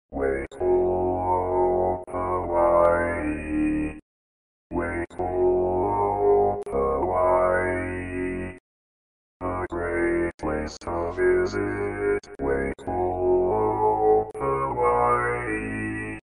How to Say or Pronounce USA Cities — Waikoloa, Hawaii
This video shows you how to say or pronounce Waikoloa, Hawaii.

A computer said Waikoloa, Hawaii. How would you say Waikoloa, Hawaii?